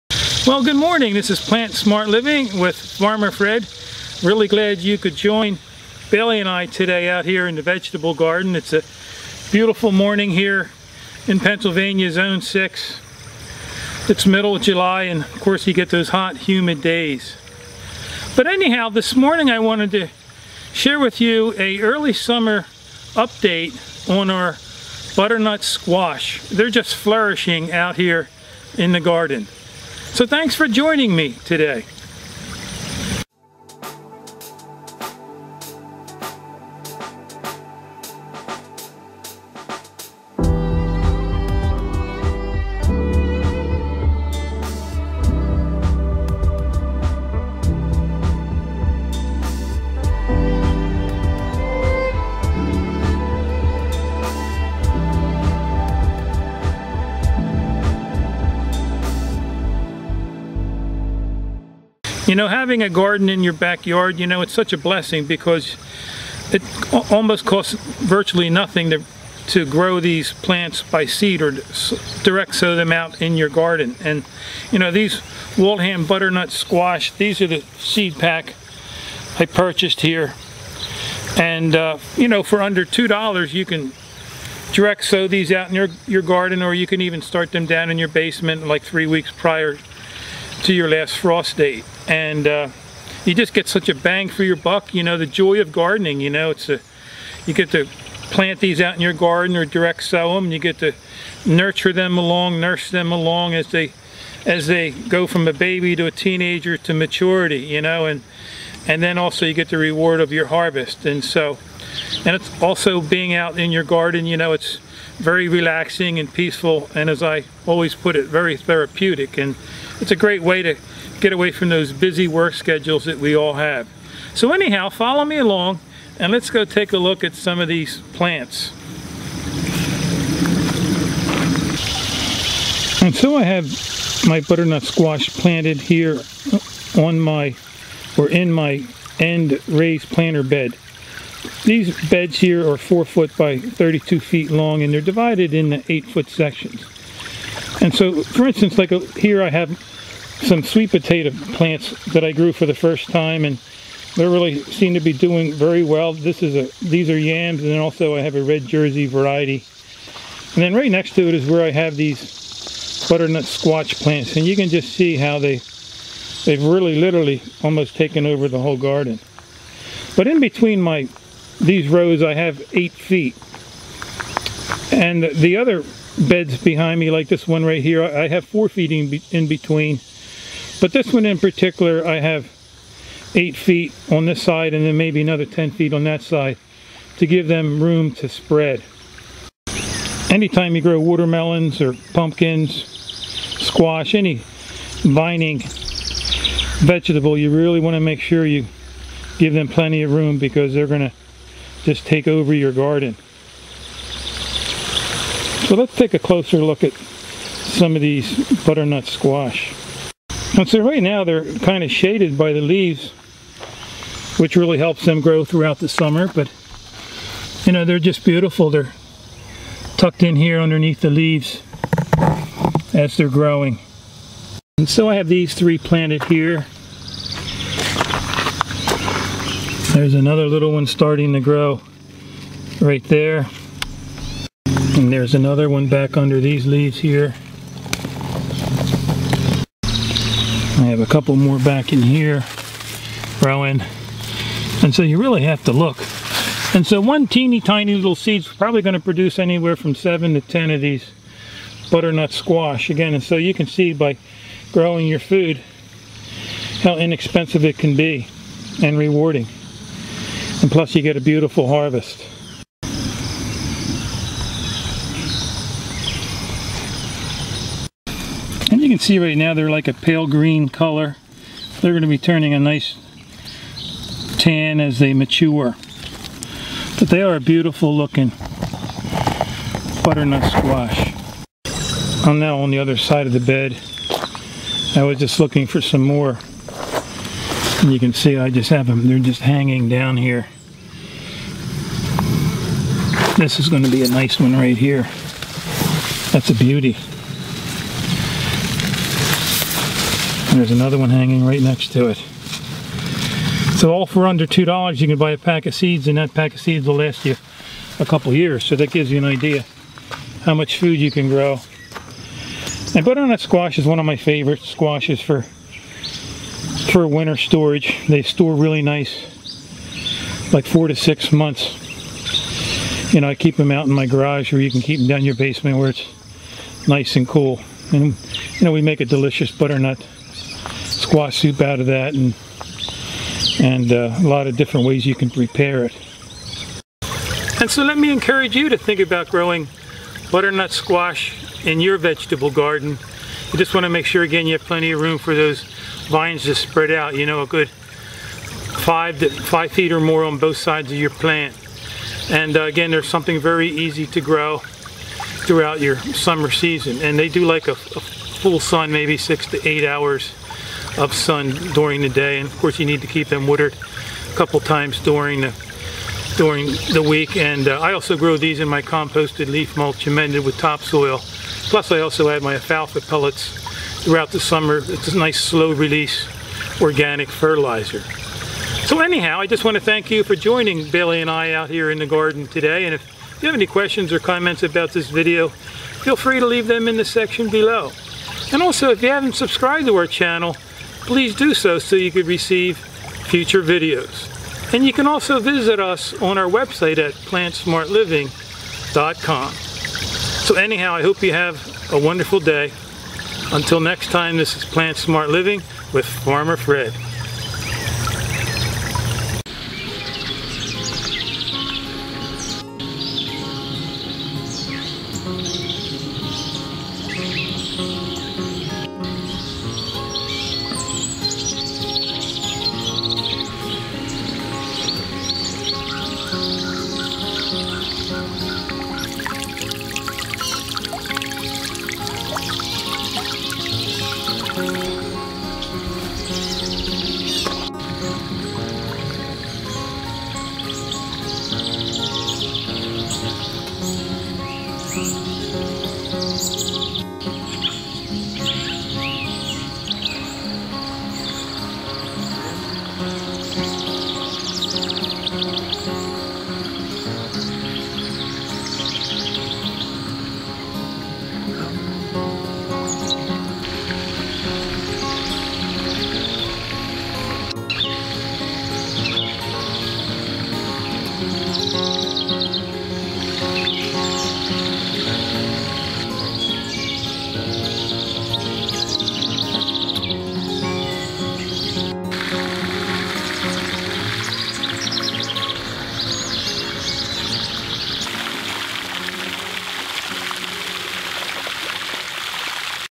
Early-Summer Garden Update: Let's Go See How the Waltham Butternut Squash Plants are Growing!
*Video Introduction Produced by My Good Friends at: (Hand & Arrow Photography Co.)

discusses the latest in Plant-Smart Living.

PlantSmartLiving ► Coming Soon!

SUPPORT
Coming Soon!